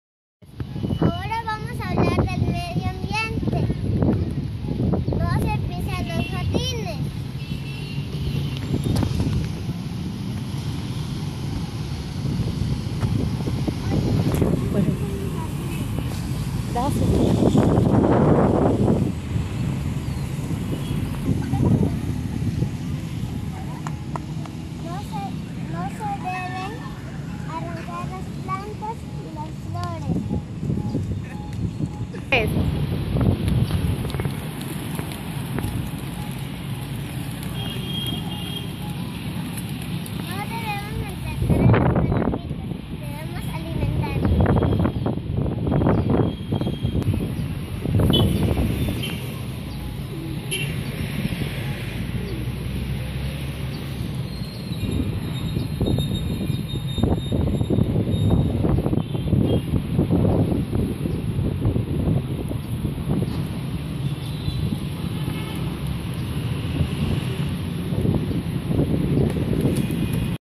El Medio Ambiente explicado por una niña
Un cortísimo video sobre el Medio Ambiente explicado por una niña